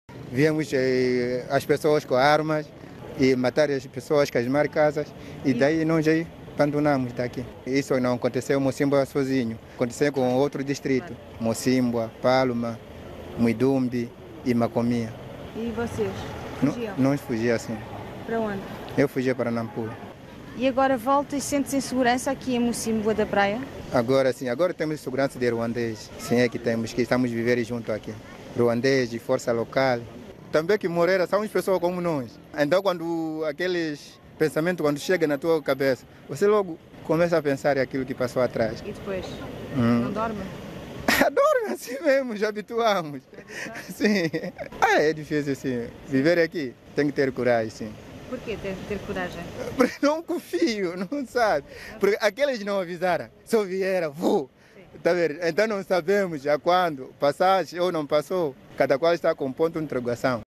CONVIDADO 11/07/23 • Cabo Delgado: "Pouco a pouco a qualidade de vida está a melhorar"
Nos últimos seis anos, estima-se que mais de 600.000 pessoas tenham fugido dos ataques do grupo terrorista islamita Shebbab, em Cabo Delgado. As populações regressam a Mocímboa da Praia e a Palma, onde "a estabilidade da região se mede através do regresso da população às zonas afectadas", afirmou o ministro da Defesa de Moçambique.